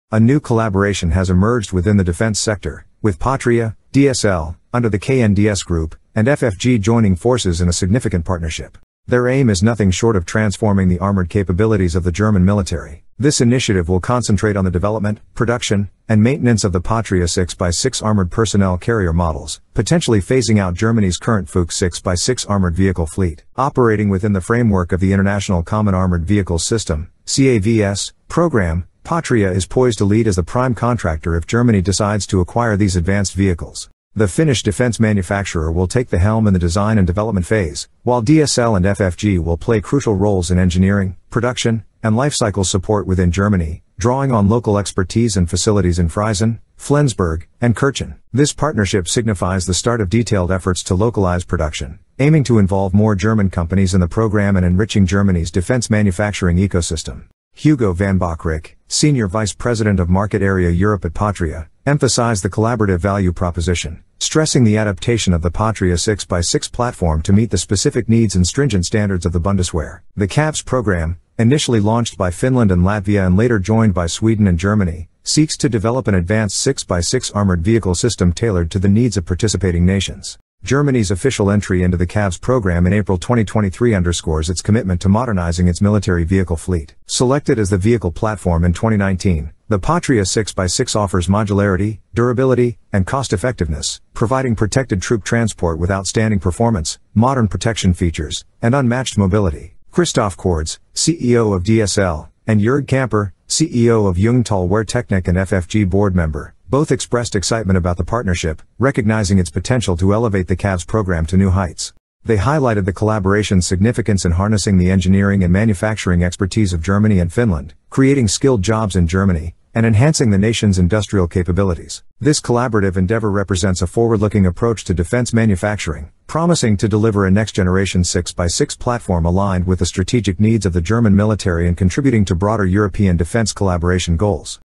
Patria from Finland, along with DSL and FFG, is replacing Germany's Fuchs armored vehicles
A new collaboration has emerged within the defense sector, with Patria, DSL (under the KNDS group), and FFG joining forces in a significant partnership. Their aim is nothing short of transforming the armored capabilities of the German military. This initiative will concentrate on the development, production, and maintenance of the Patria 6x6 armored personnel carrier models, potentially phasing out Germany's current FUCHS 6x6 armored vehicle fleet. Operating within the framework of the international Common Armoured Vehicles System (CAVS) program, Patria is poised to lead as the prime contractor if Germany decides to acquire these advanced vehicles. The Finnish defense manufacturer will take the helm in the design and development phase, while DSL and FFG will play crucial roles in engineering, production, and life-cycle support within Germany, drawing on local expertise and facilities in Freisen, Flensburg, and Kirchen.